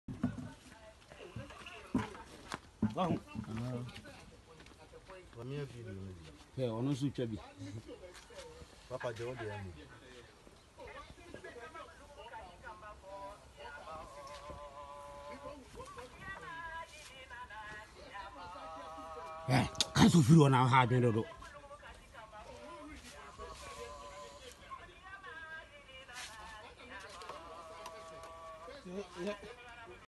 Soldiers catch live python about to slaughter for food
Video from BA4